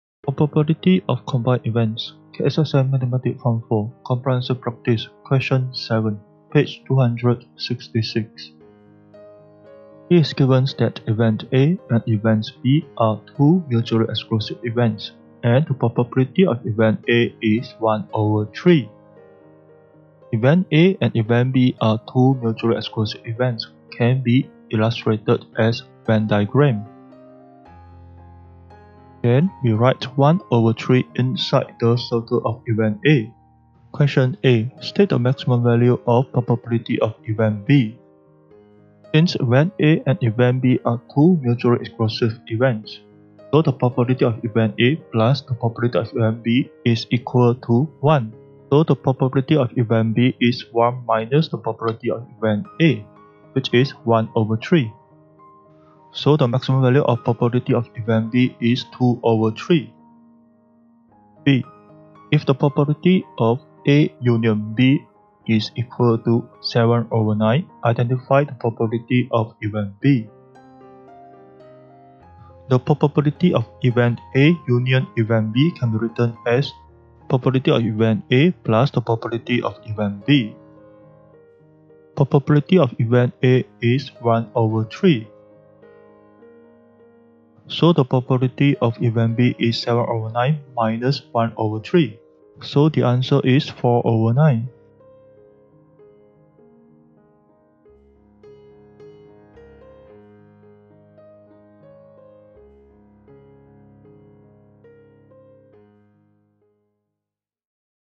Comprehensive Practice - Question 7 - Math Form 4 Chapter 9
KSSM Mathematics Form 4
Probability of  Combined Events
Comprehensive Practice
Question 7
Page 266